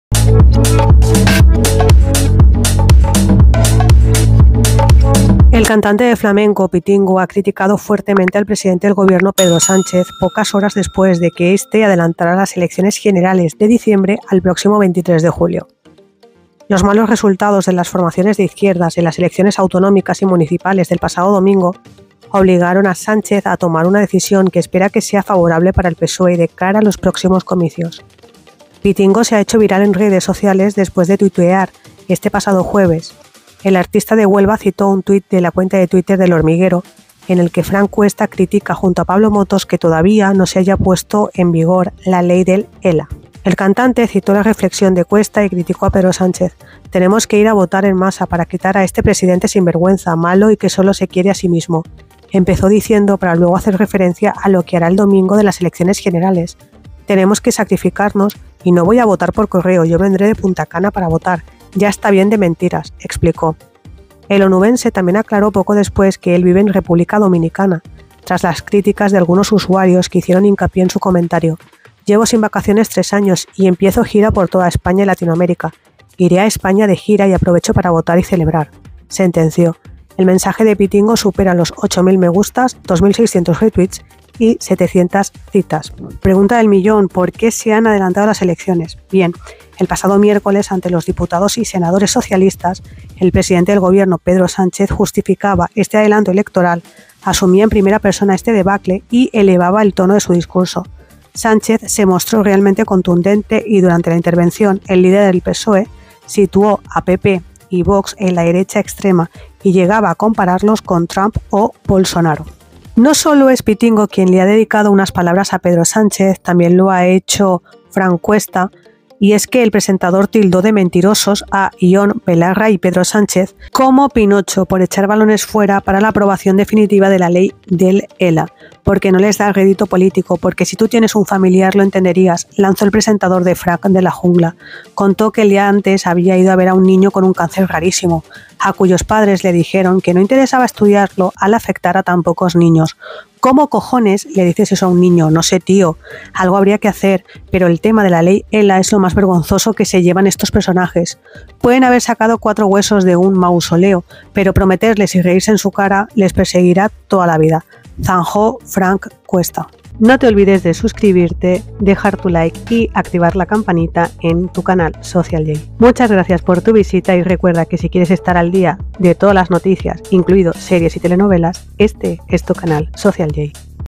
Pitingo carga contra Sánchez y anima a «quitar a este presidente sinvergüenza»
elecciones generales
pedro Sánchez